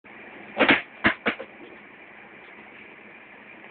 Train Madness 06
Things to do when your in a train (Nokia 6230)